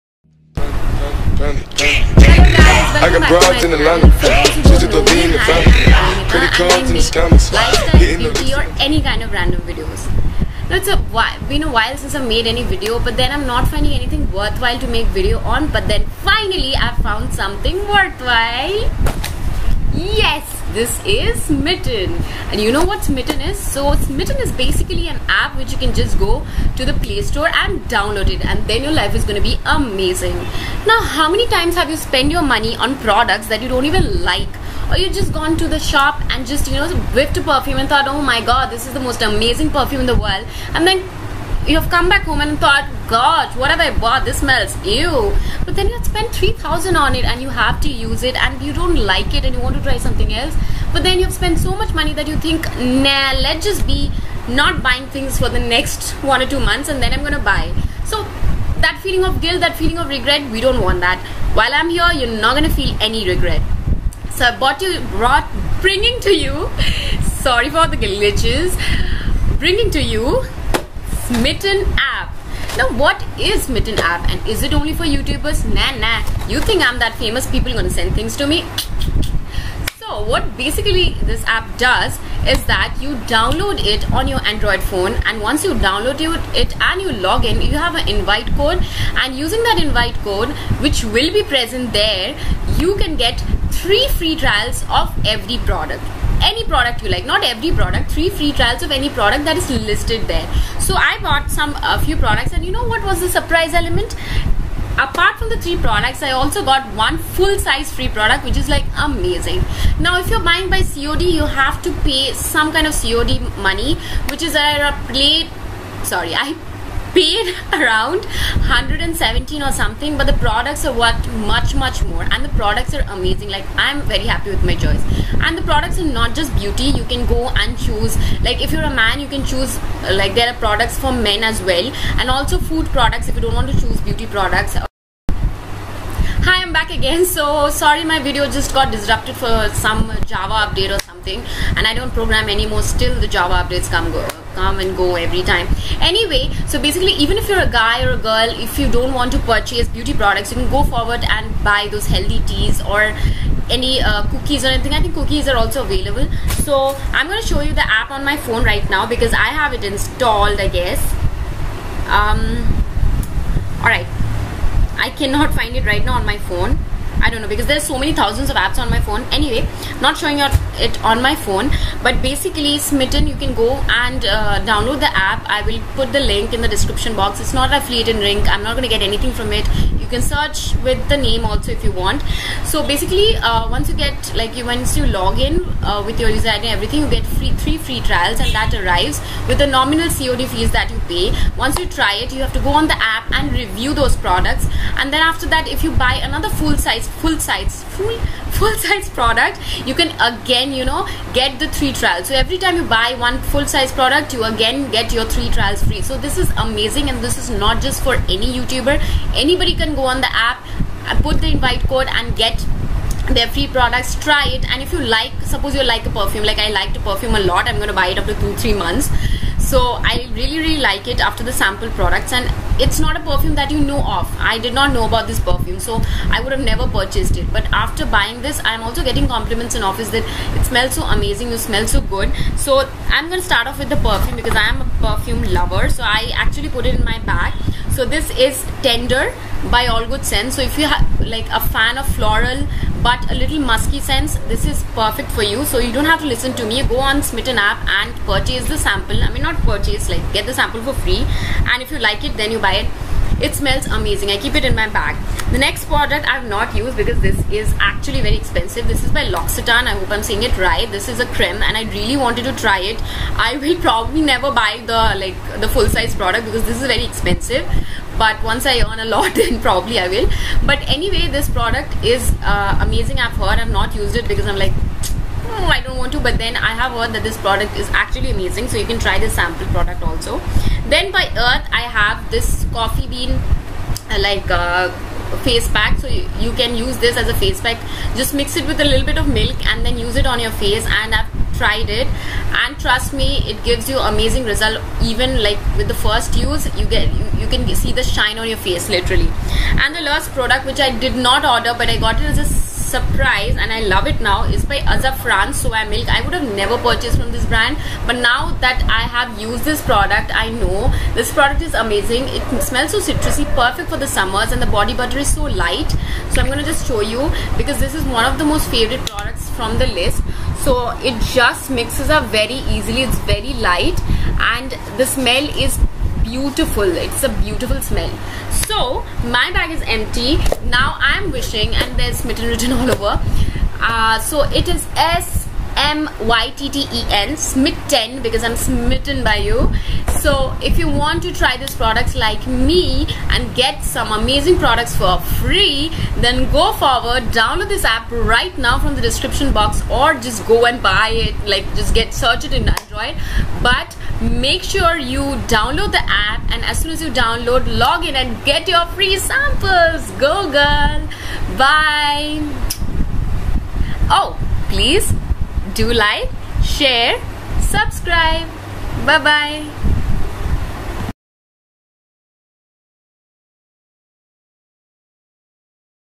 ||Get free Samples Of Luxury Products||
Smytten is an amazing app which lets you try free samples of luxury items so that you donot waste your money buying full size samples that you donot like
Available for both Android and Iphone

Send your products for review,just drop a mail
Thanx lovlies